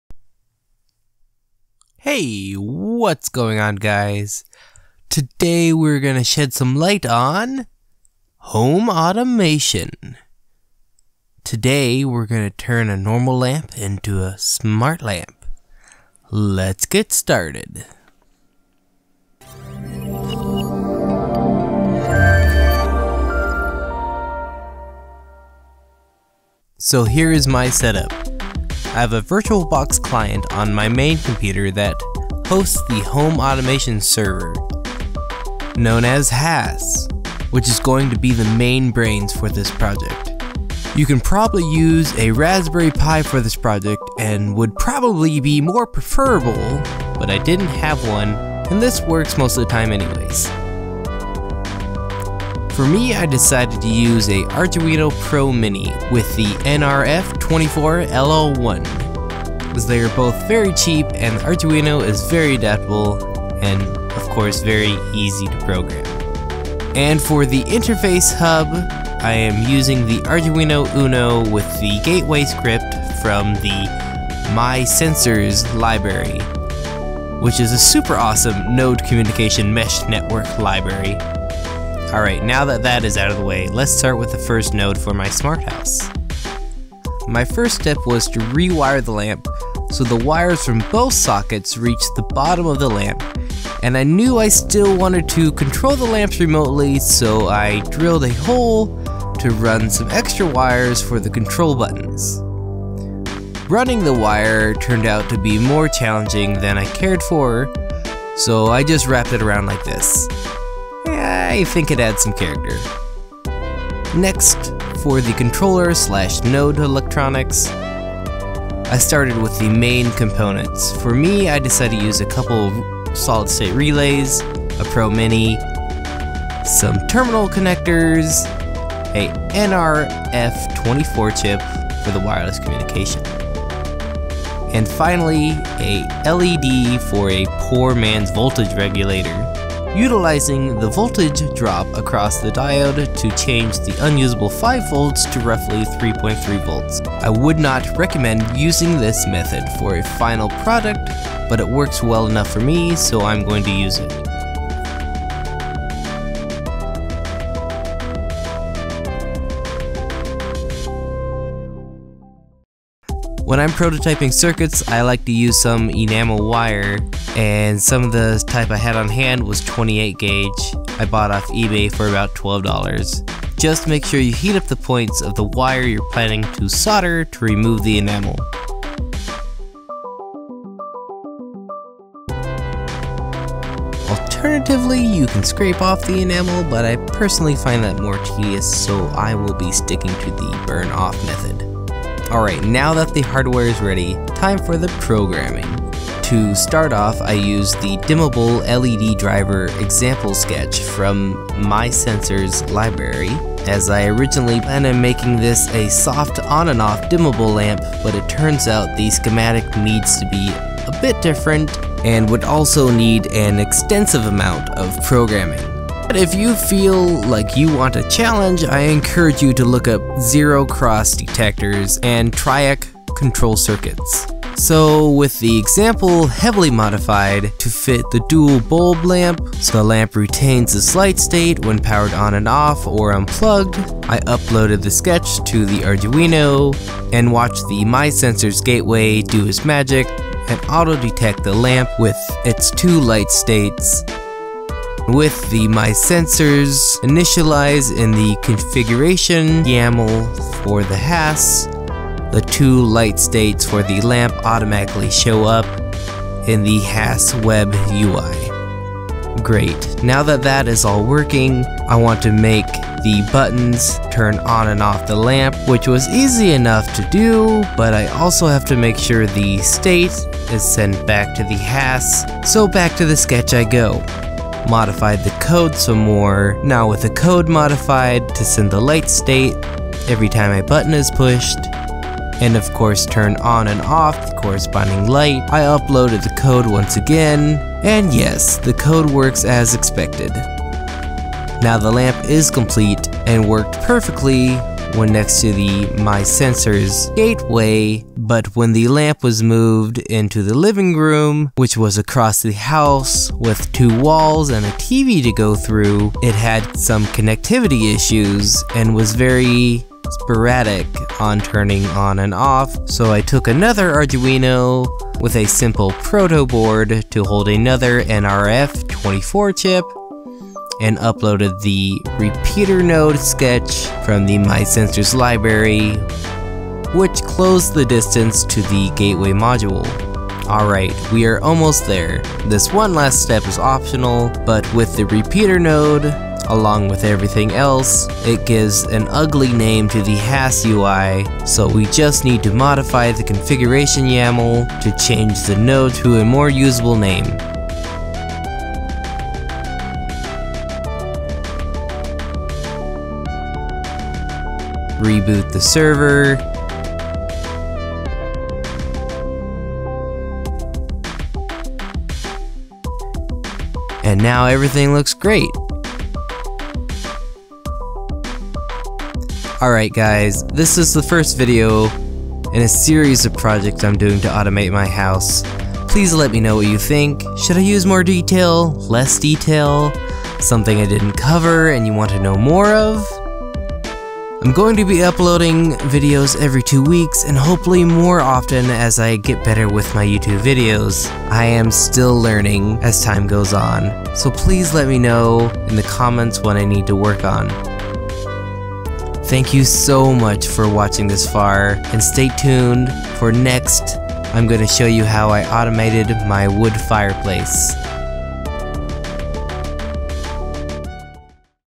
H.A: Part 1 DIY Smartlamp
This video is the first of my Home Automation series using the Home Assistant Server, MySensors Library and the power of Arduinos

You will learn how I made my DIY Smartlamp it was a very fun project

Please let me know if you find this interesting or what i could of done better Im still very new at this.